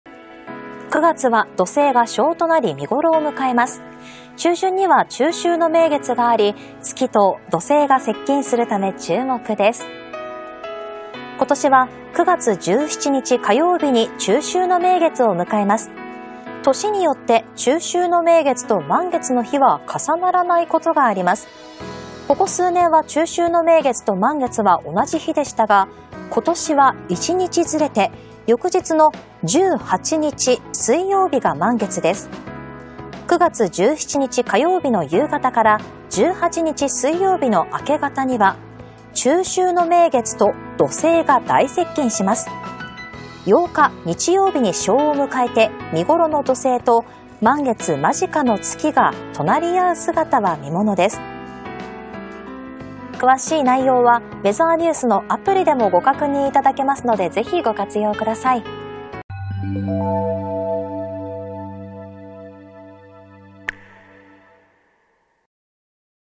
【9/17・中秋の名月!!】見頃の土星にも注目
今年は9月17日(火)に「中秋の名月」を迎えます。年によって中秋の名月と満月の日は重ならないことがあります。ここ数年は中秋の名月と満月は同じ日でしたが、今年は1日ずれて翌日の18日(水)が満月です。

中秋の名月とは、月の満ち欠けをもとに1か月の日付を決める太陽太陰暦（旧暦）の8月15日の夜に見える月のことです。

中秋の名月を愛でる習慣は平安時代に中国から伝わったとされており、日本では中秋の名月は農業の行事と結びついて「芋名月」と呼ばれることもあります。

次に中秋の名月がちょうど満月となるのは、2030年9月12日になります。